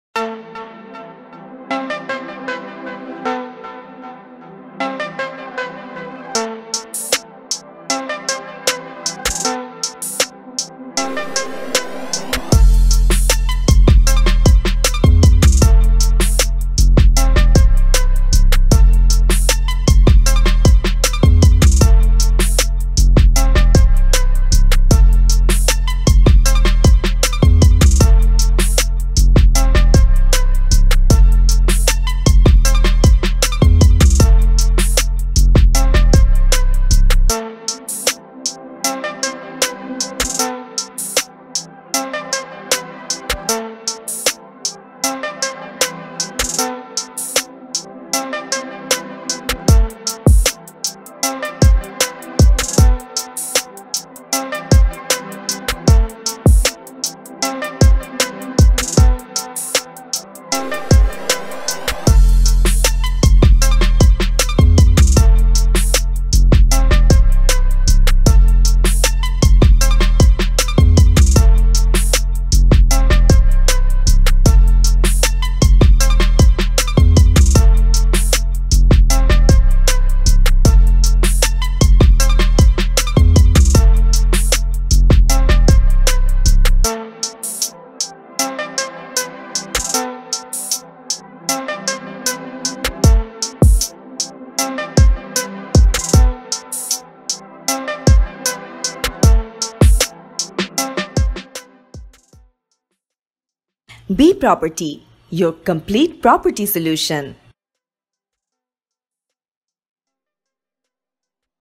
Well Planned 1,050 sft Flat for Rent in Mirpur - Dhaka
Mirpur was originally built as a residential area which has kept its good name for a while now. There are happy residents and plenty of houses in the area. The 1,050 sft house features all rooms in a neatly arranged order. Every essential facility comes with the apartment and you will find that nothing is left out.